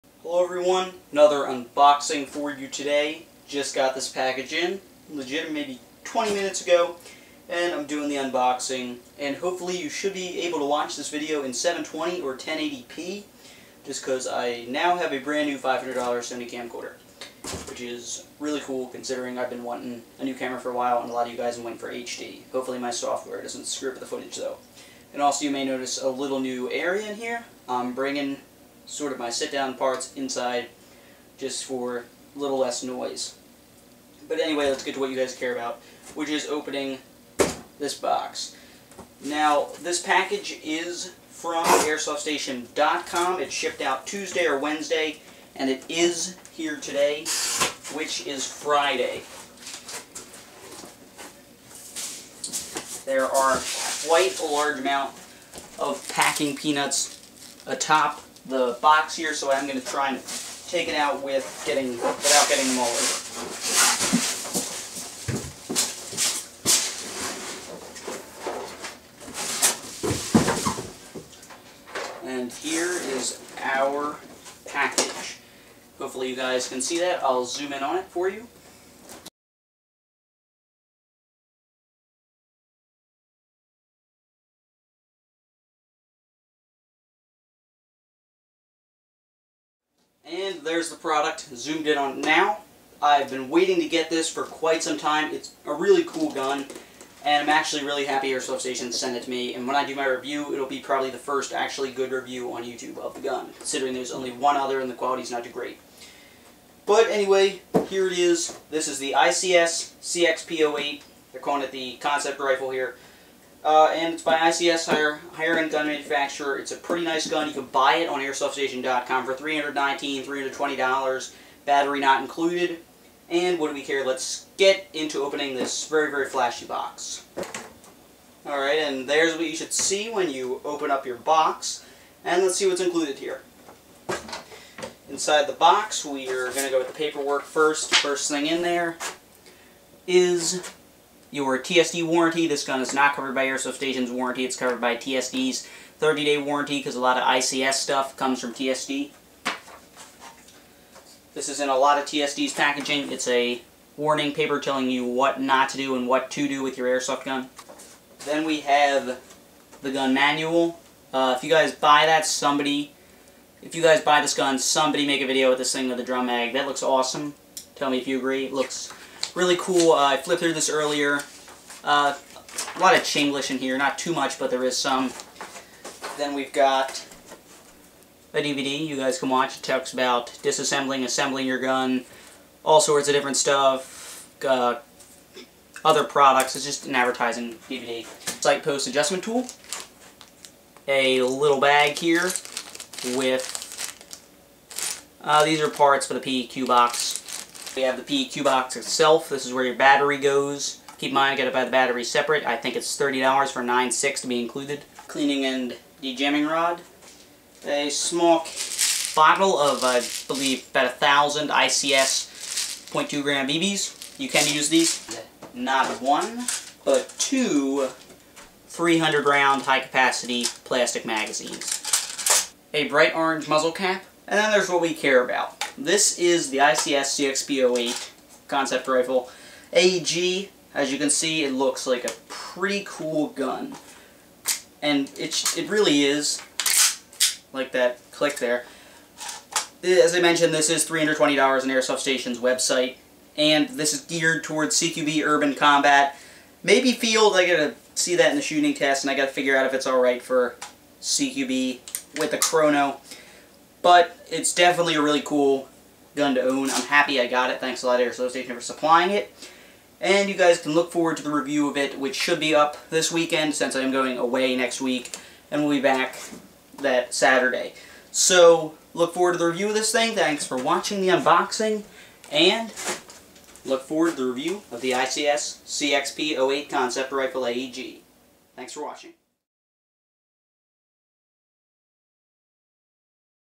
ICS CXP.08 Airsoft AEG Unboxing
An unboxing video of the ICS CXP.08 Airsoft AEG